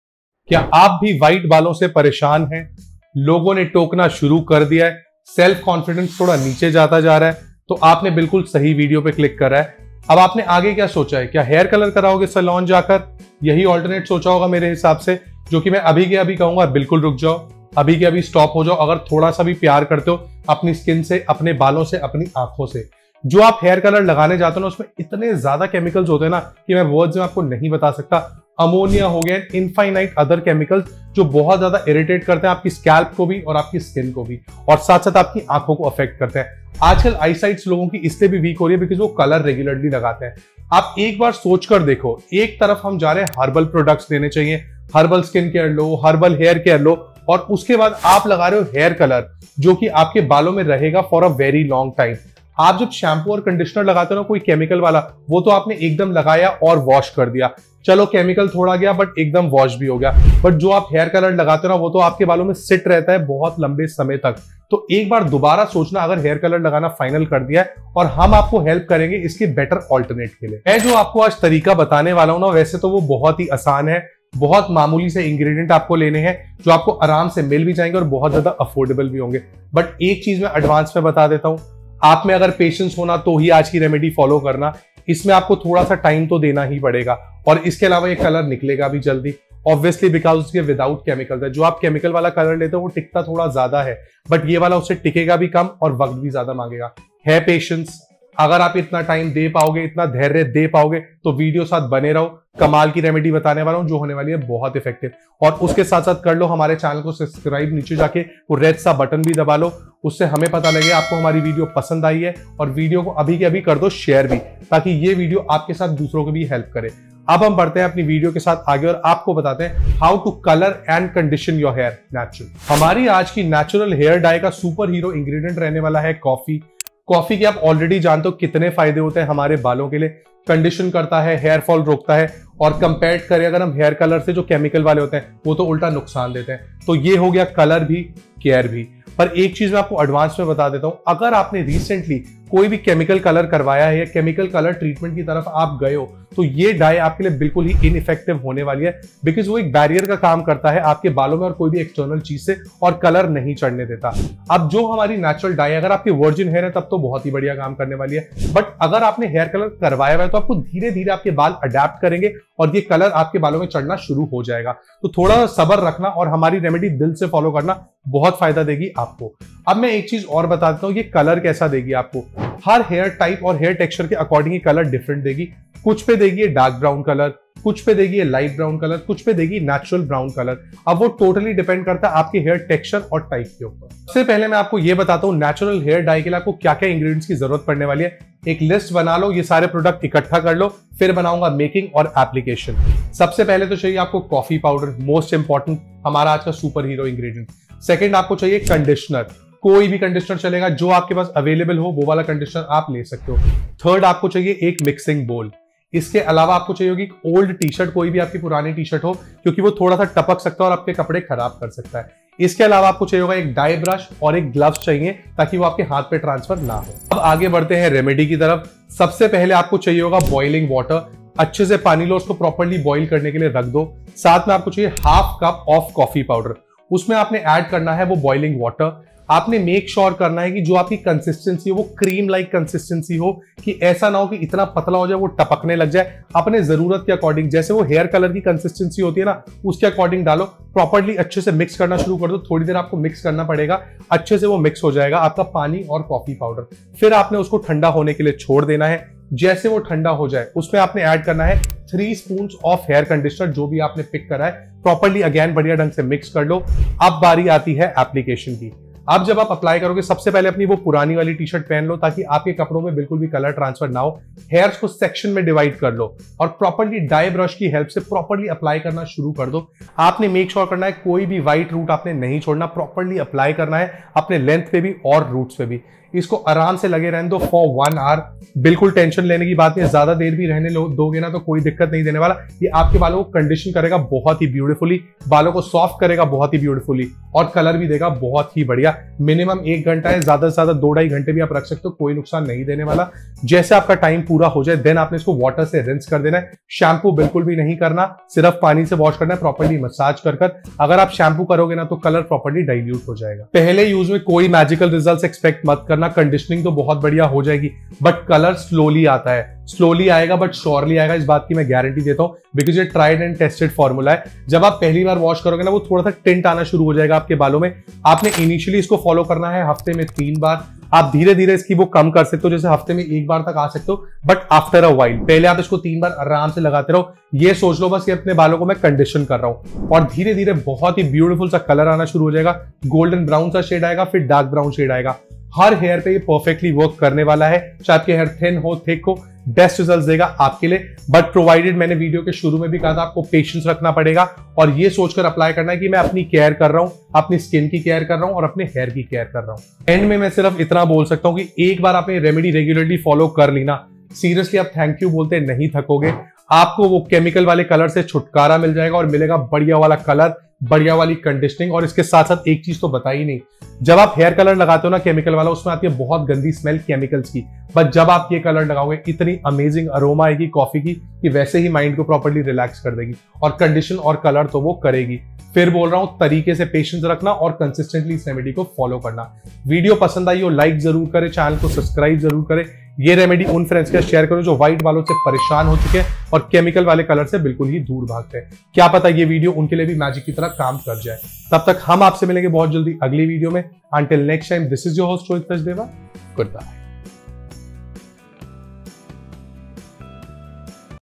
सफेद बालों को जड़ से Condition & Color करो || 100% Natural Hair Dye
In Today's video we will talk about a natural hair dye that will give you natural Color, Conditioning & also boost hair growth.